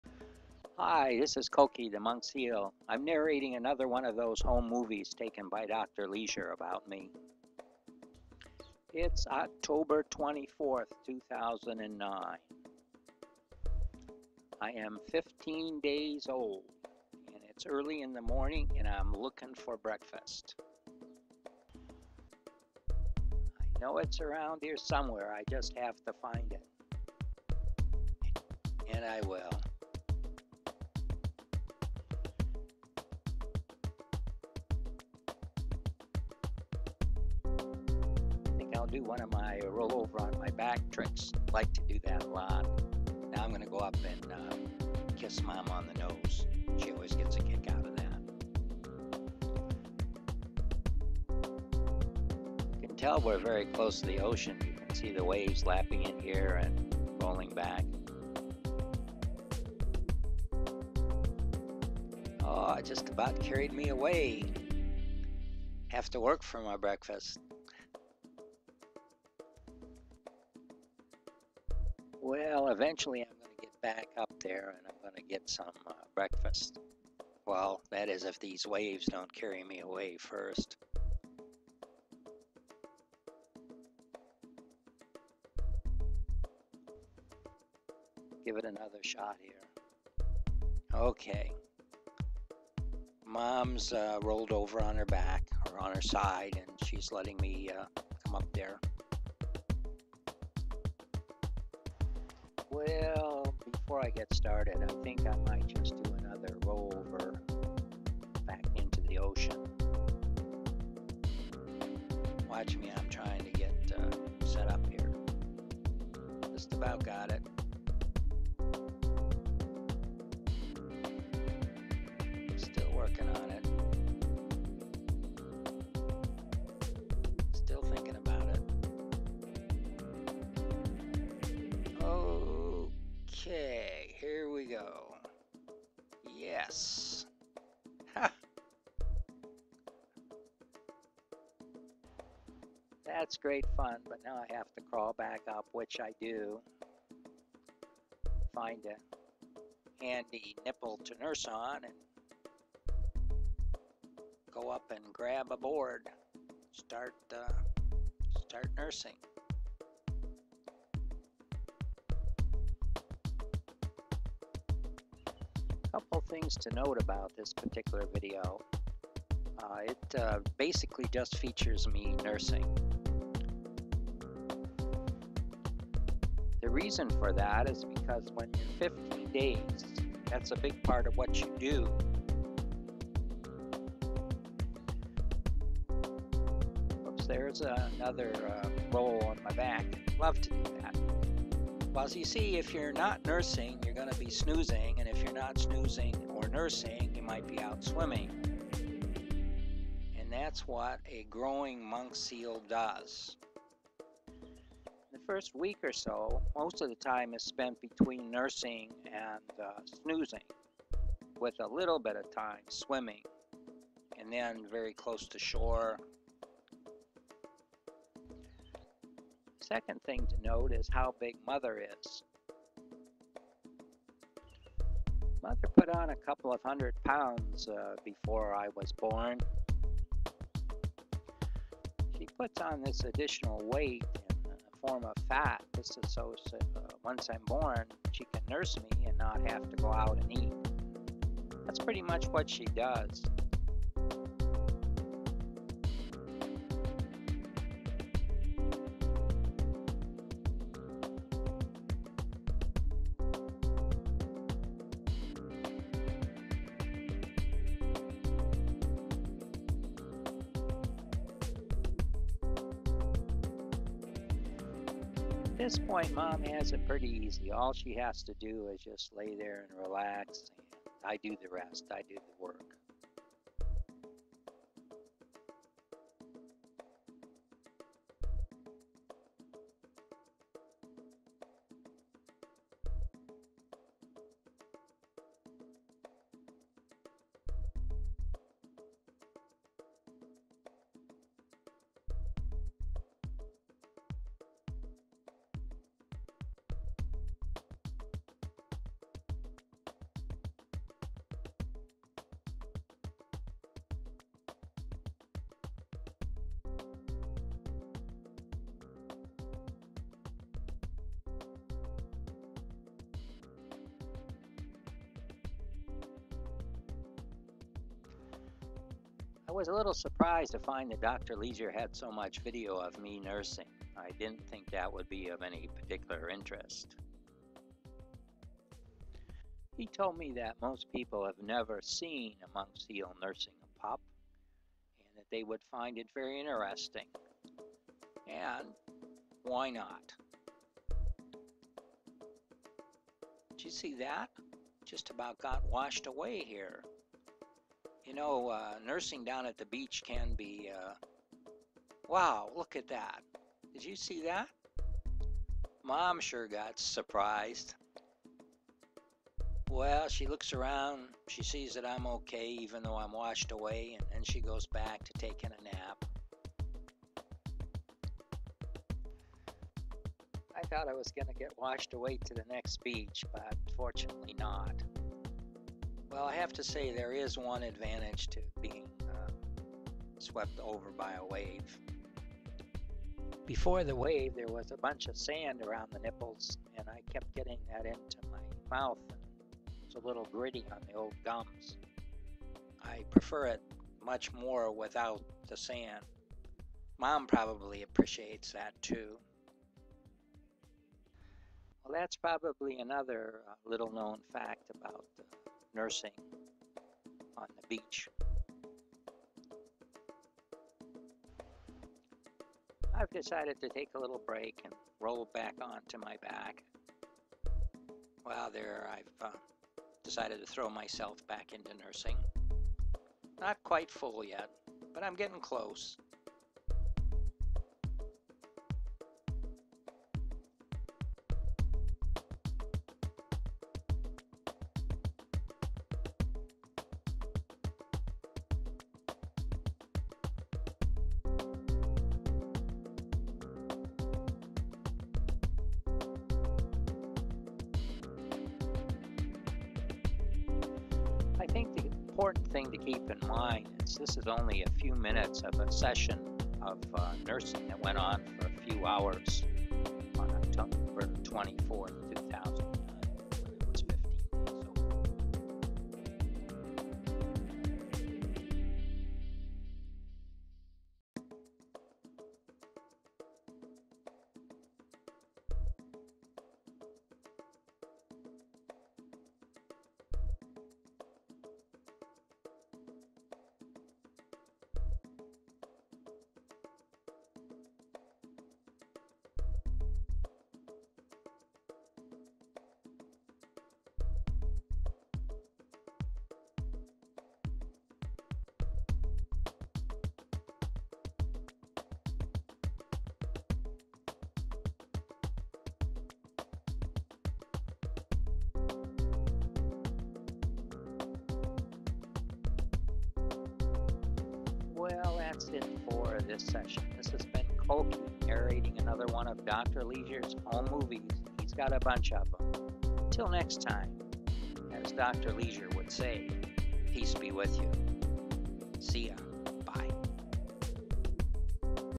Koki Monk Seal Documentary - Breakfast Tsunami
Home movie Documentary of Koki the Monk Seal taken October 24, 2009 by Dr. Leisure. Narrated by Koki himself! Koki is fifteen days old in the video. Features having breakfast at the beach. Highlights some of the unique challenges to having breakfast at Koki Beach Park outside Hana, Hawaii. Problems of sand in the milk  and mini tsunami waves.